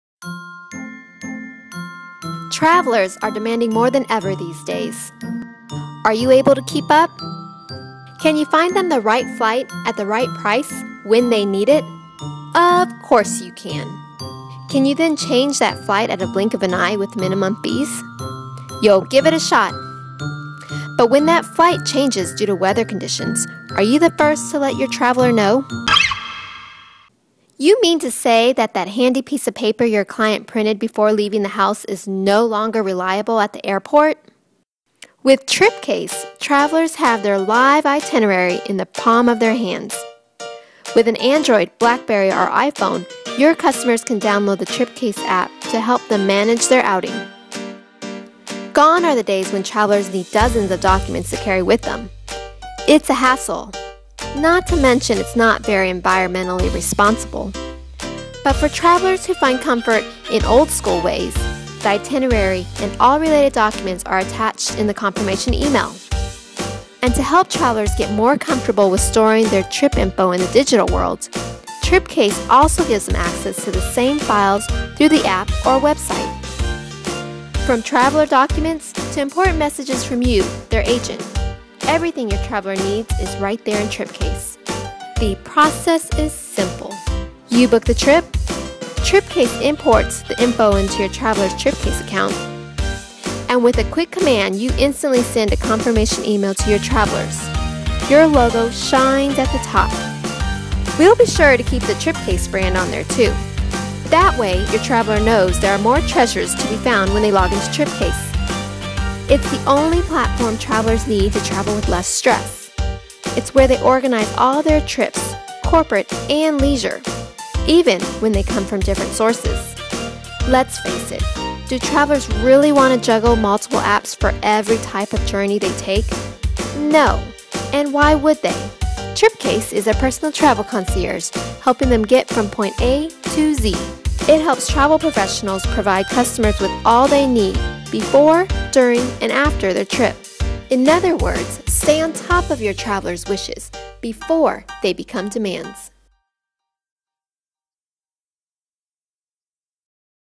TripCase
Take the stress out of your customers' travel experience by equipping them with TripCase®, part of the Sabre® Red™ Service suite. It's the only traveller service platform your business needs to help your customers travel with confidence and less stress.